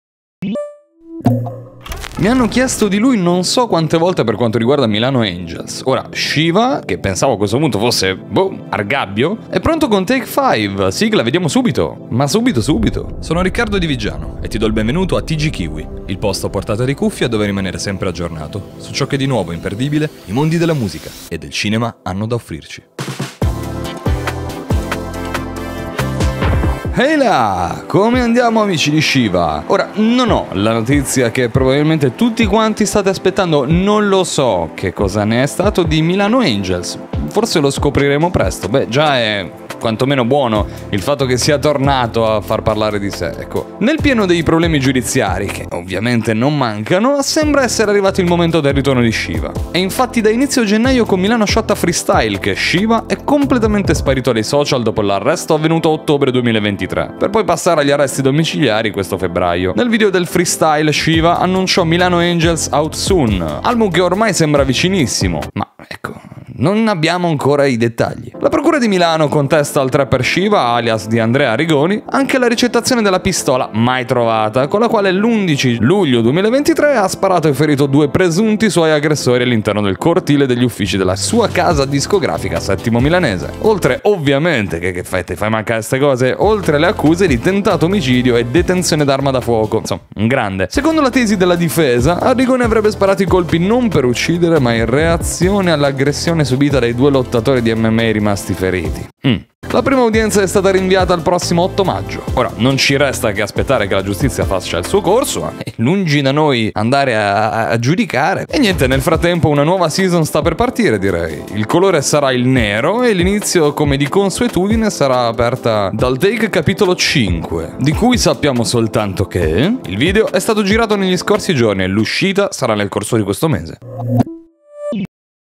TAKE 5 - SHIVA (MILANO ANGELS)
Nel pieno dei problemi giudiziari, sembra essere arrivato il momento del ritorno di Shiva. Una nuova season è in arrivo...

Animazioni: Andrea Gatto
Voce: Riccardo Diviggiano
Autore: Andrea Simone Galbussera
Sonoro: Samuele D'Elia (TLR)